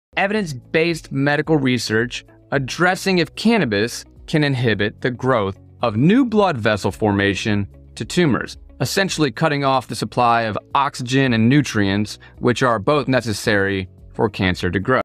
THC CAN HELP CANCER!?
Dr. Henke is Discussing Cannabis and THC- The potential role of THC in modern neuro-oncology. specifically discussing the potential to decrease the vascular supply to tumors.

Widespread communication and education is how we create change.🙏